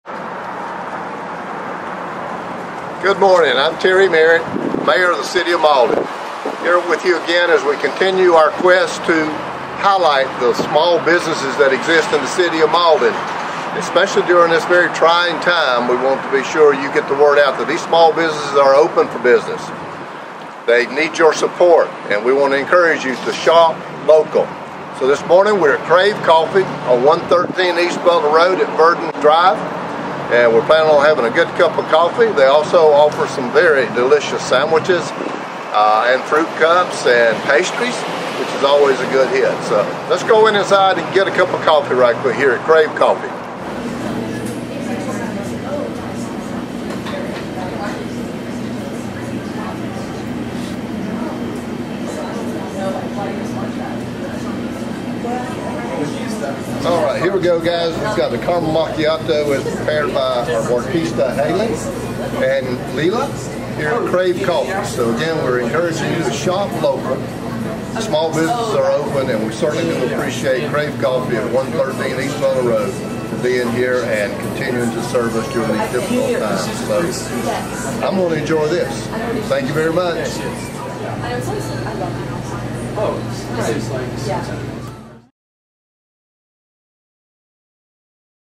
ShopSmall in Mauldin: Crave Coffee
During its November 16, 2020 meeting, Mayor Merritt and the members of City Council proclaimed November 28, 2020 as Small Business Saturday and urged the residents of our community, and communities across the country, to support small businesses and merchants on Small Business Saturday and throughout the year. Mayor Terry Merritt is highlighting local businesses in this shopsmall video series. Please continue to support small businesses and merchants on Small Business Saturday and throughout the year!